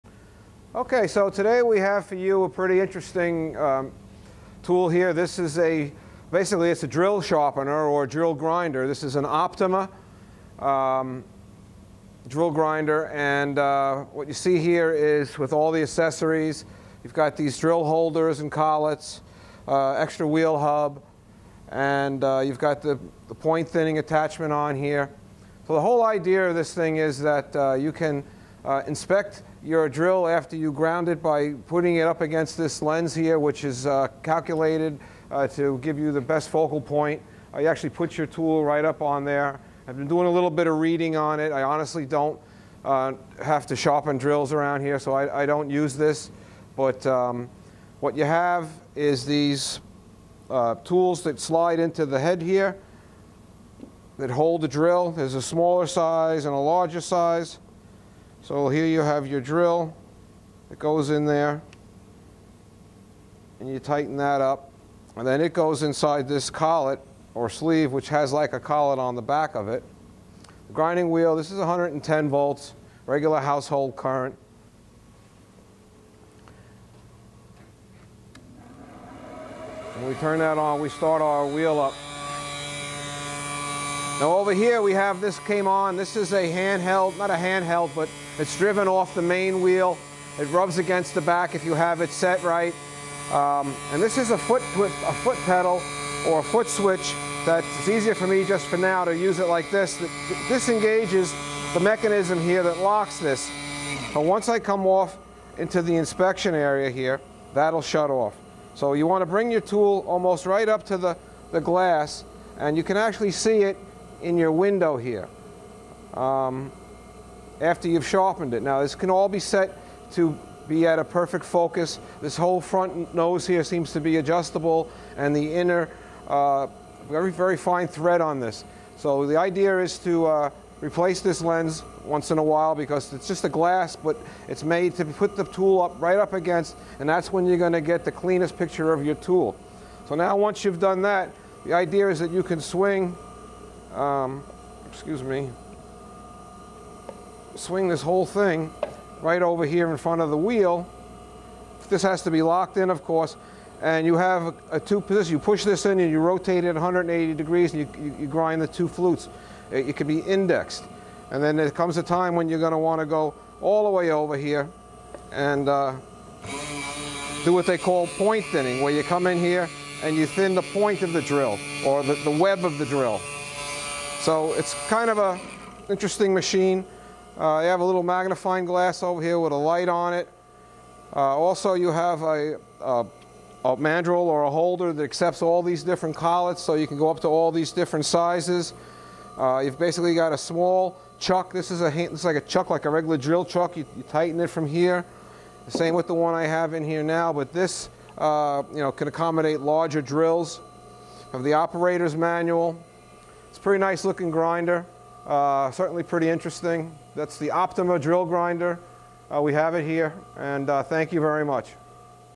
OPTIMA (MADE IN SWITZERLAND) HIGH PRECISION DRILL GRINDER
MODEL: UNIVERSAL     S/N: B176.65     NEW: 1990's

RANGE. .0135"- 1"DIAMETER
POINT ANGLE. 70 DEGREES TO 145 DEGREES &
           (DIFFERENT SETTING) 145 DEGREES TO 180 DEGREES
PRIMARY CUTTING ANGLE. 0 DEGREES TO 40 DEGREES
SECONDARY CLEARANCE ANGLE. 0 DEGREES TO 40 DEGREES
POINT THINNING WHEEL. 6800 RPM

APPROX. WEIGHT (MACHINE ONLYo 120 LBS.

STANDARD FEATURES:
CAPABILITY TO GRIND RIGHT AND LEFT HAND DRILLS, HIGH SPEED
  STEEL AND CARBIDE TIPPED OR SOLID CARBIDE DRILLS, SPADE
  DRILLS, COUNTERSINKS, CENTER DRILLS OF ALL TYPES, TWO
  AND THREE FLUTED DRILLS AND END MILLS (THREE FLUTED
  OPTIONAL), TAPER SHANK DRILLS NO. 2 AND 3, SLOW OR FAST
  SPIRAL, (UP TO 3/4" DIAMETER ANY LENGTH, STANDARD LENGTH
  ABOVE 3/4" DIAMETER).
  MACHINE WILL GRIND AND POINT THIN OR SPLIT POINT IN ONE
  SETTING.
BUILT-IN OPTICAL COMPARATOR WITH MAGNIFYING LENS
BUILT-IN DIAMOND WHEEL DRESSER
BUILT-IN POINT SPLITTING OR WEB THINNING ATTACHMENT
FOOT PEDAL
SINGLE PHASE ELECTRICS

EQUIPPED WITH:
VERY NICE COMPLIMENT OF ACCESSORIES AS SHOWN IN THE PHOTOS

** EXCELLENT CONDITION & APPEARANCE (ORIGINAL PAINT) **
** ARRIVING SOON TO OUR HARRISON, NJ WAREHOUSE **